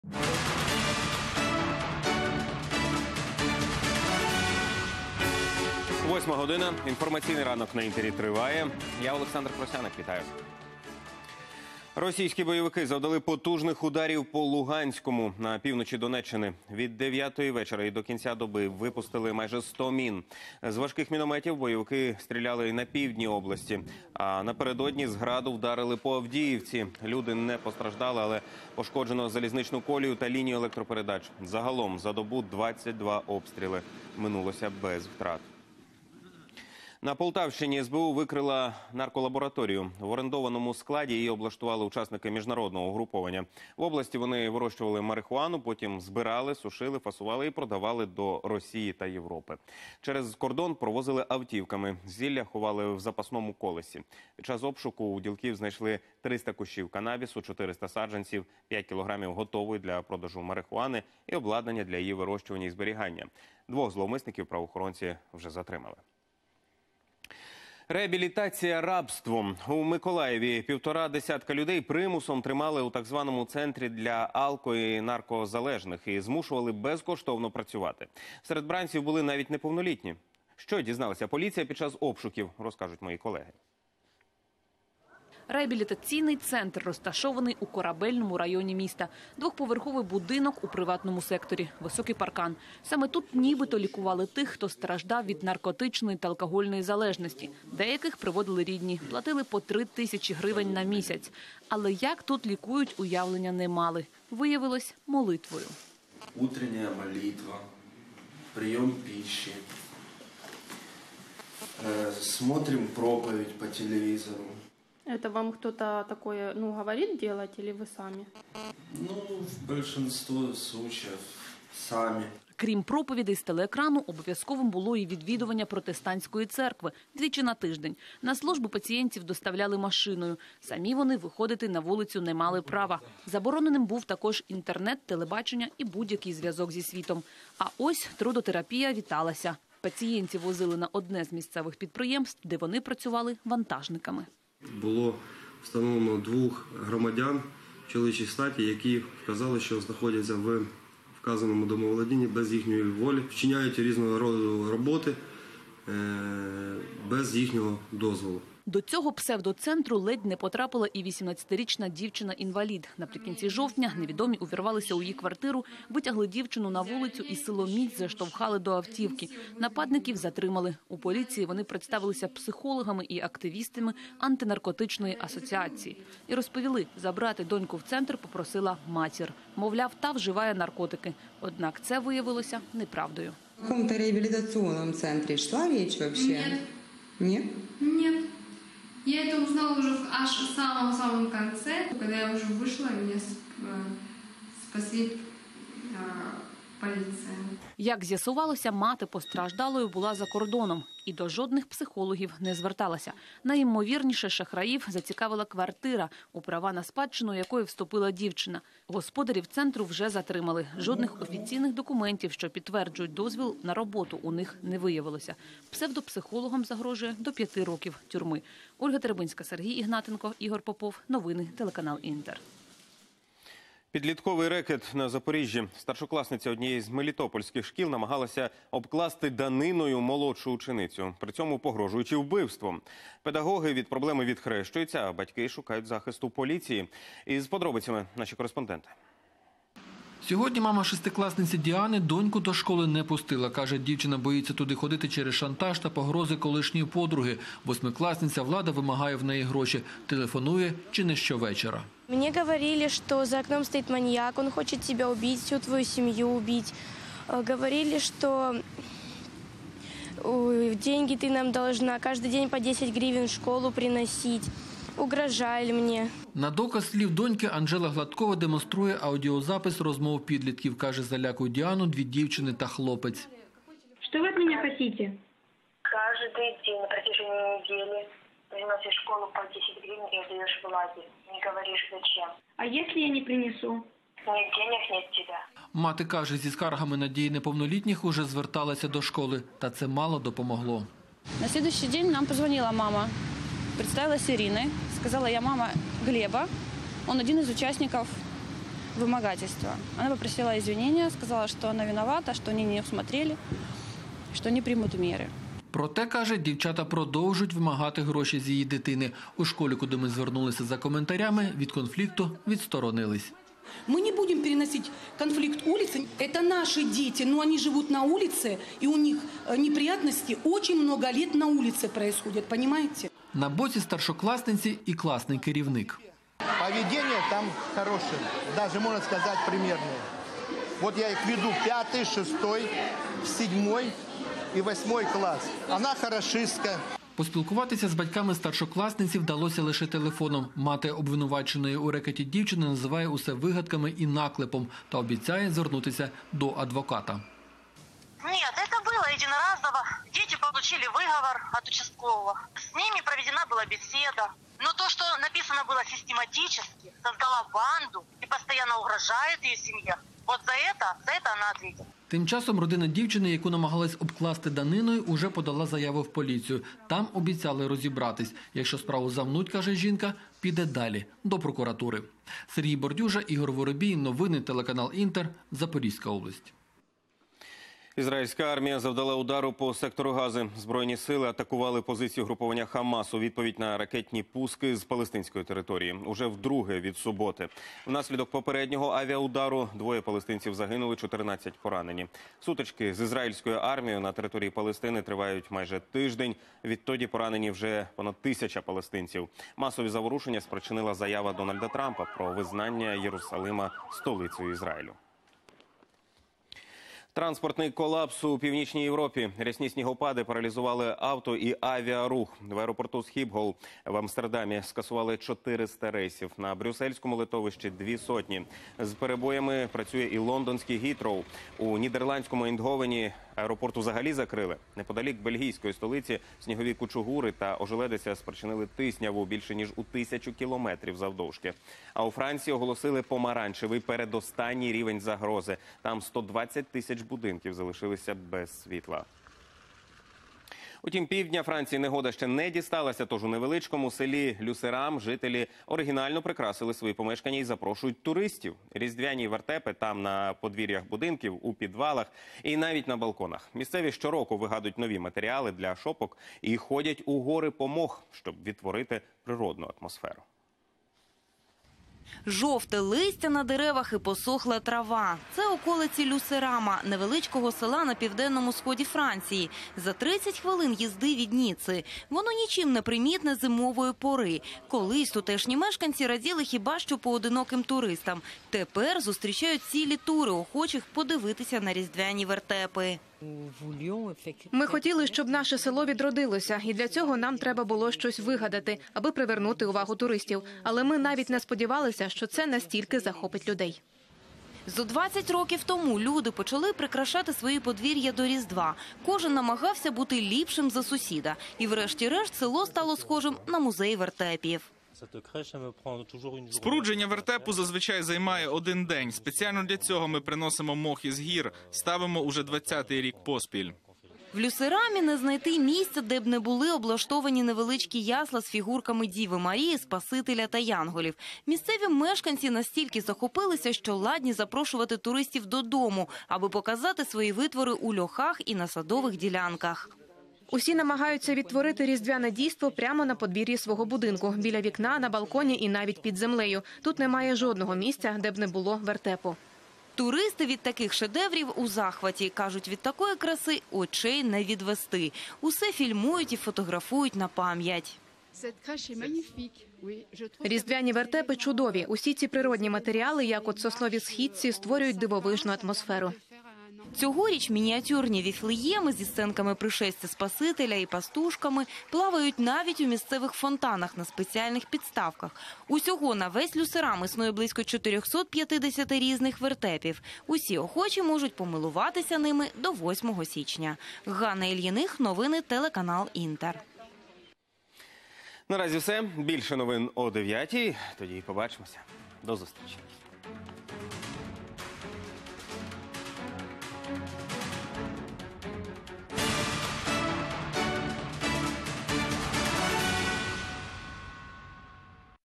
Новости 8:00, выпуск за 12 декабря 2017 года
Головні події на цю годину: Окупанти продовжують використовувати на Донбасі заборонену зброю; На Полтавщині ділки створили наркокартель; Реабілітація рабством: у Миколаєві шахраї створили псевдо-центр для наркозалежних; У Мелітополі старшокласниця обклала даниною ученицю молодших класів; Ізраїльська армія завдала удару по сектору Гази; Негода у Європі: снігопади паралізували авто- та авіа- транспорт; Жителі Франції підготували для туристів Різдвяні вертепи.